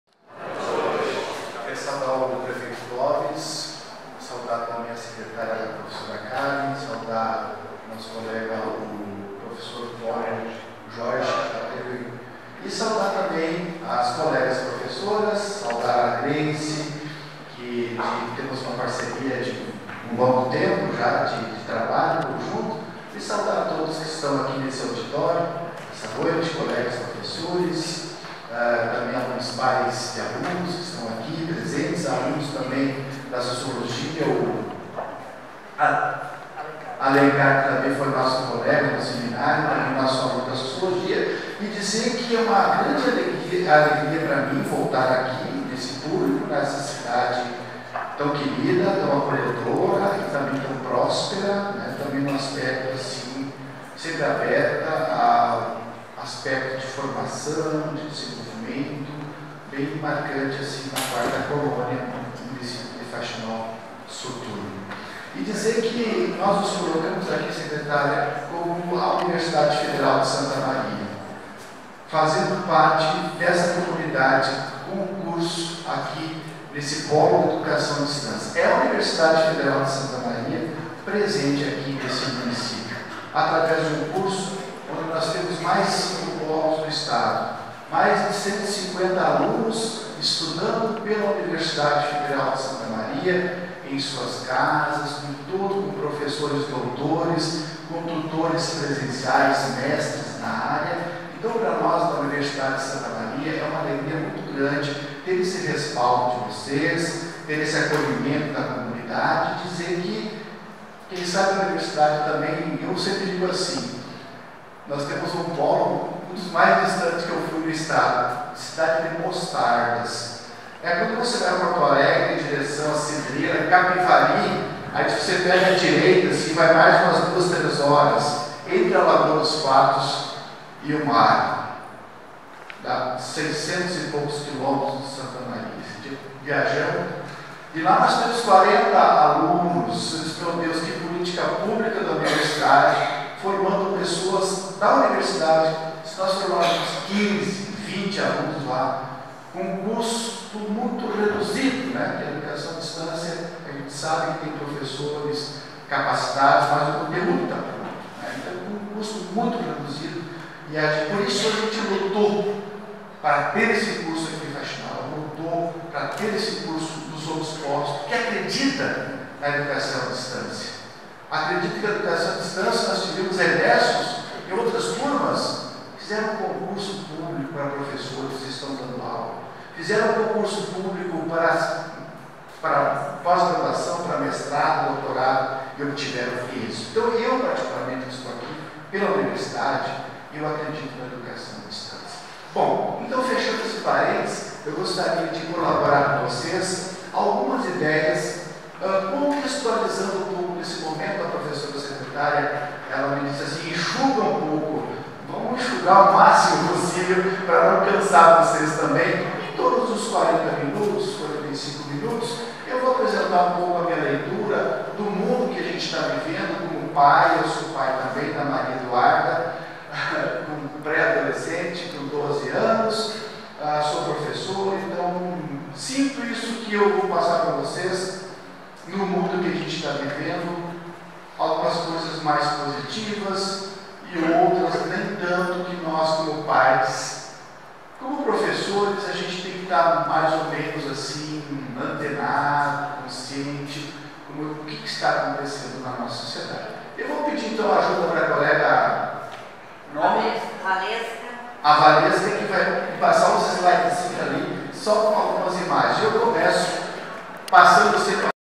Família e Educação na Pós-modernidade
Introdução: A extensão da Universidade na Comunidade. Professor Dejalma Cremonese faz palestra sobre Família e Educação na Pós-modernidade aos professores e pais de alunos da rede municipal de ensino de Faxinal do Soturno - RS.